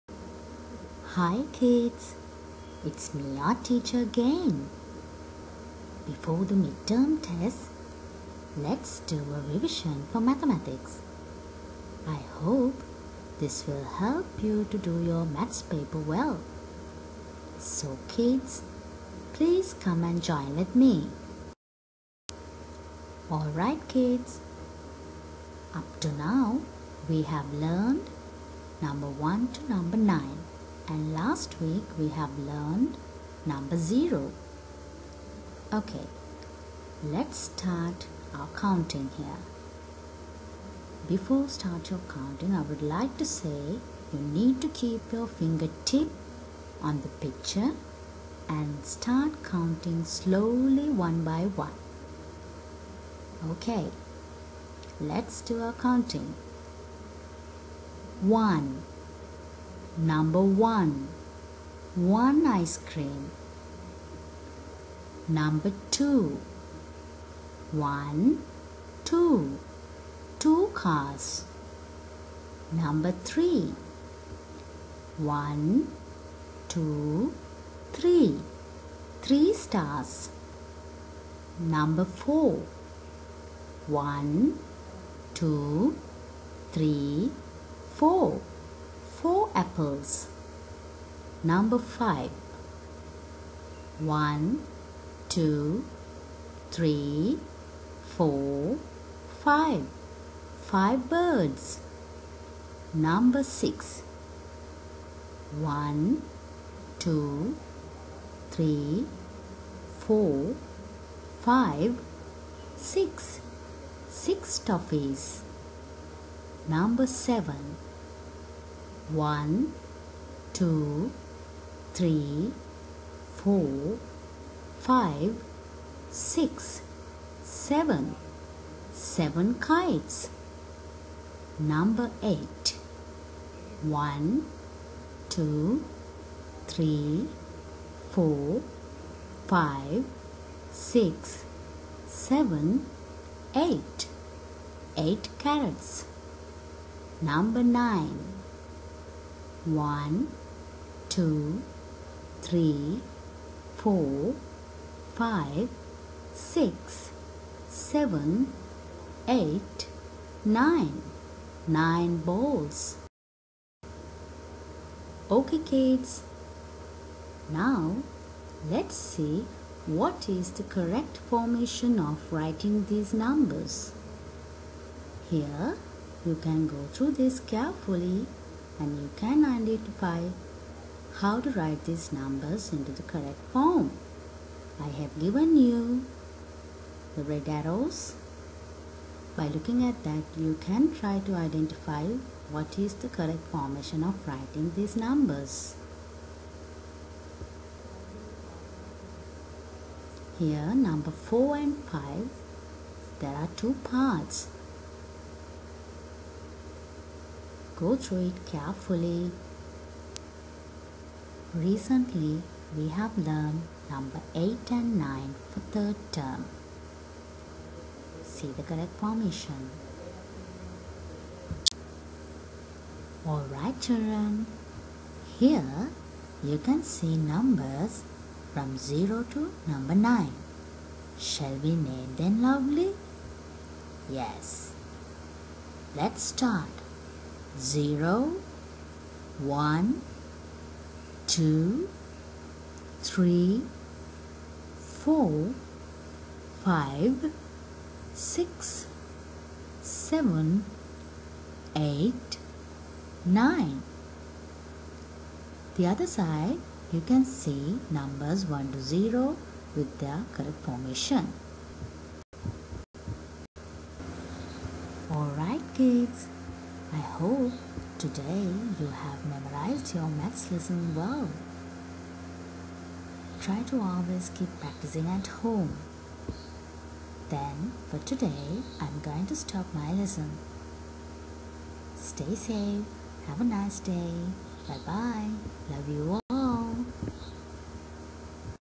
Mathematics - Revision 1-9 for Nursery & Primary kids
Number 1-9 counting with pictures, correct formation of writing 1-9.